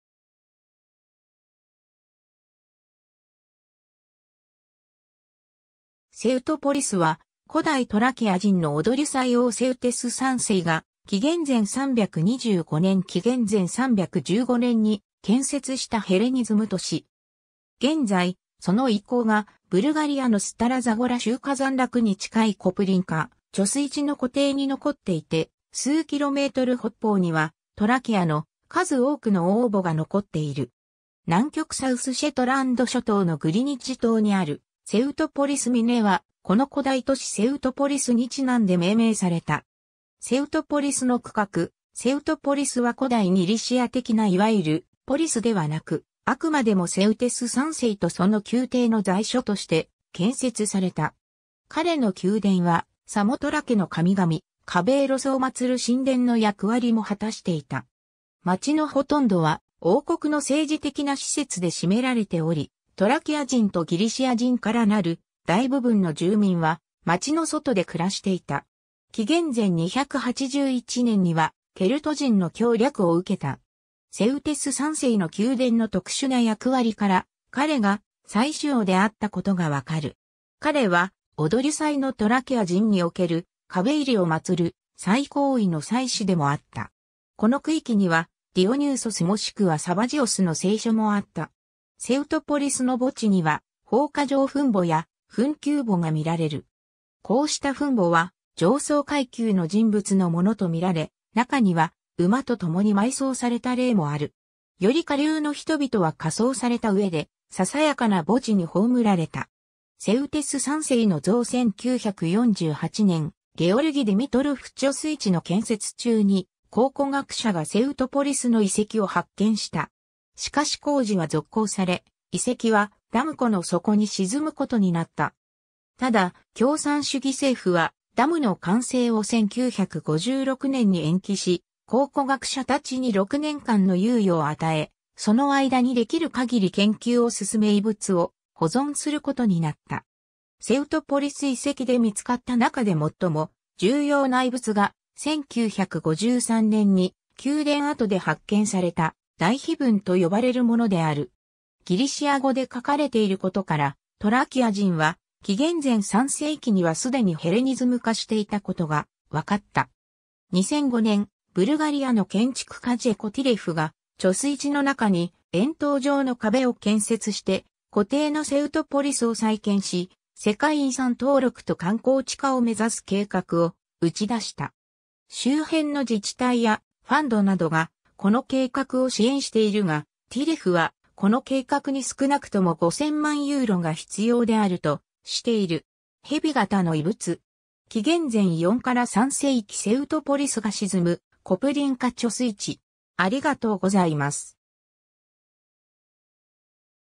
セウトポリス
スタラ・ザゴラ州
セウトポリス (古代ギリシア語: Σευθόπολις) は、古代トラキア人のオドリュサイ王セウテス3世が、紀元前325年-紀元前315年に建設したヘレニズム都市。
現在、その遺構がブルガリアのスタラ・ザゴラ州カザンラクに近いコプリンカ貯水池の湖底に残っていて、数キロメートル北方には、トラキアの数多くの王墓が残っている。
南極サウス・シェトランド諸島のグリニッジ島にあるセウトポリス峰は、この古代都市セウトポリスにちなんで命名された。
 セウトポリスの区画 セウトポリスは古代ギリシア的ないわゆる「ポリス」ではなく、あくまでもセウテス3世とその宮廷の在所として建設された。
彼の宮殿は、サモトラケの神々カベイロスを祀る神殿の役割も果たしていた。
街のほとんどは王国の政治的な施設で占められており、トラキア人とギリシア人からなる大部分の住民は街の外で暮らしていた。
紀元前281年にはケルト人の強掠を受けた。
 セウテス3世の宮殿の特殊な役割から、彼が祭祀王であったことがわかる。
彼はオドリュサイのトラキア人における、カベイリを祀る最高位の祭司でもあった。
この区域には、ディオニューソスもしくはサバジオスの聖所もあった。
 セウトポリスの墓地には、蜂窩状墳墓や墳丘墓がみられる。
こうした墳墓は上層階級の人物のものとみられ、中には馬とともに埋葬された例もある。
より下流の人々は火葬されたうえで、ささやかな墓地に葬られた。
 セウテス3世の像 1948年、ゲオルギ・ディミトロフ貯水池（後に「コプリンカ」に改名）の建設中に、考古学者がセウトポリスの遺跡を発見した。
しかし工事は続行され、遺跡はダム湖の底に沈むことになった。
ただ共産主義政府は、ダムの完成を1956年に延期し、考古学者たちに6年間の猶予を与え、その間にできる限り研究を進め遺物を保存することになった。
 セウトポリス遺跡で見つかった中で最も重要な遺物が、1953年に宮殿跡で発見された「大碑文」と呼ばれるものである。
ギリシア語で書かれていることから、トラキア人は紀元前3世紀には既にヘレニズム化していたことが分かった。
 2005年、ブルガリアの建築家ジェコ・ティレフが、貯水池の中に円筒状の壁を建設して湖底のセウトポリスを再建し、世界遺産登録と観光地化を目指す計画を打ち出した。
周辺の自治体やファンドなどがこの計画を支援しているが、ティレフはこの計画に少なくとも5000万ユーロが必要であるとしている。
 蛇型の遺物。
紀元前4-3世紀 セウトポリスが沈むコプリンカ貯水池